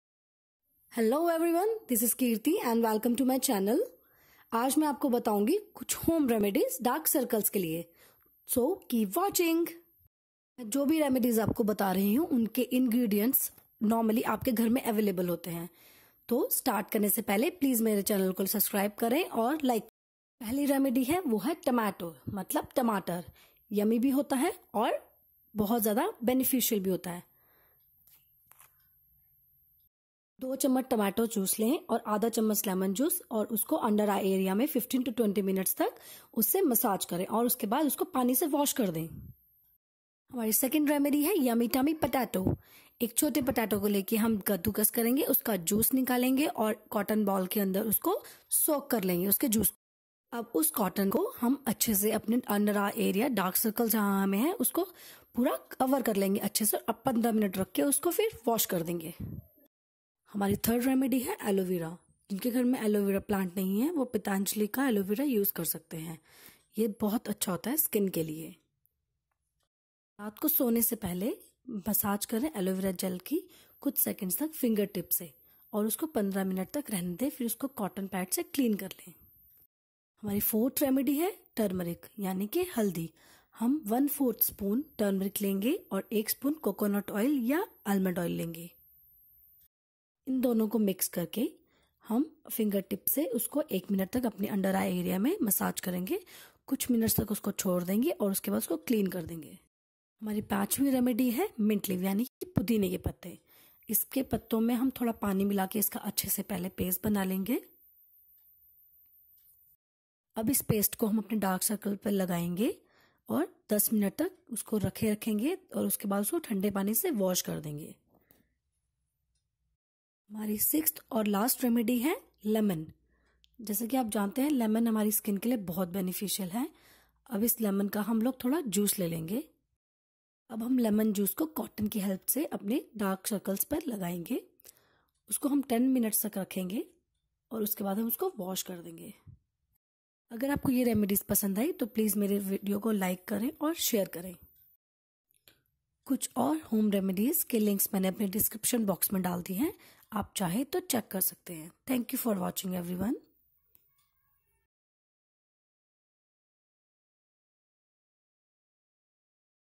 Top 6 home remedies for dark circles ||Remove Dark Circles at home || ingredients easily available
Check out some home remedies for dark
circles under eyes problem with natural ingredients.
No side effects of natural ingredients.

THANKS
KIRTI